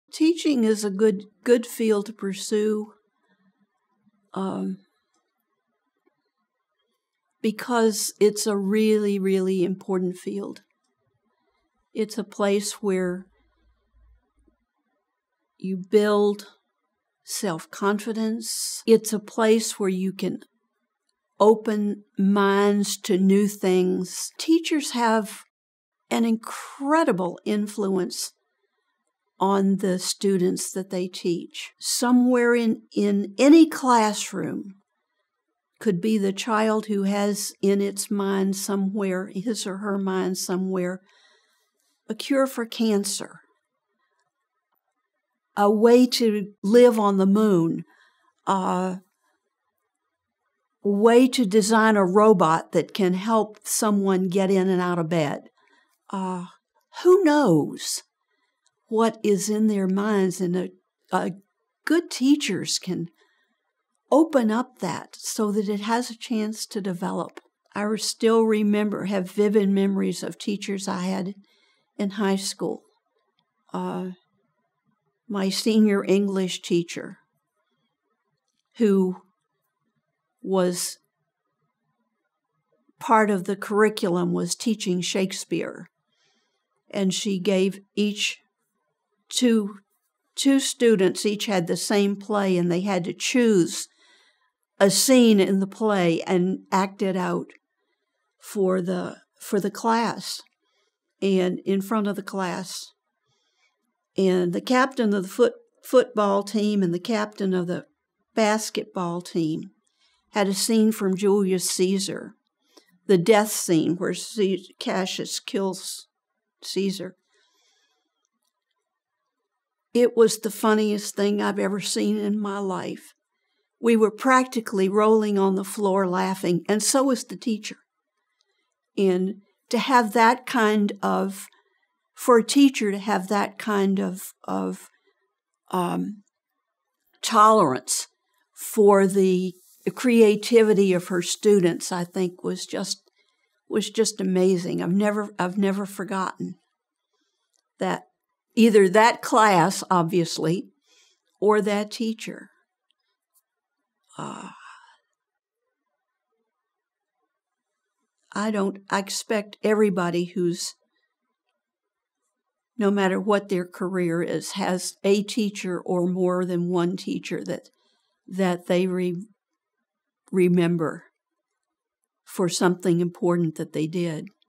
Why Should Girls Consider Careers in Education | Master Teacher Nancy Andrews | Career Girls
Watch Nancy Andrews, a retired Master Teacher in literacy with the State of Maine Department of Education.